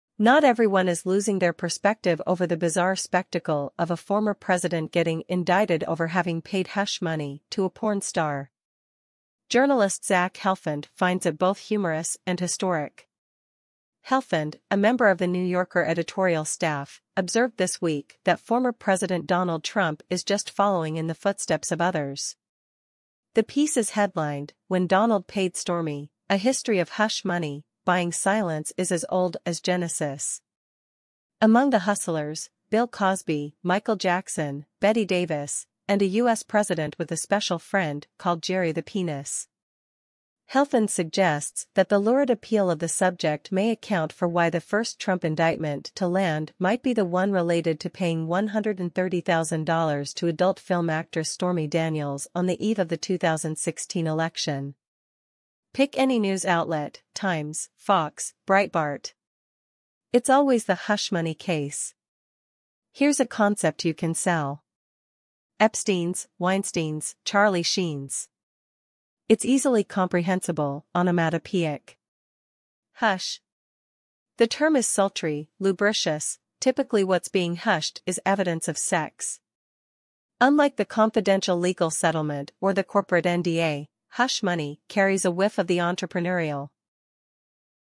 Trump's hush money payment is part of a rich tradition dating back to Genesis
Despite the bizarre spectacle of a former president being indicted for paying hush money to a porn star, not everyone is losing their perspective.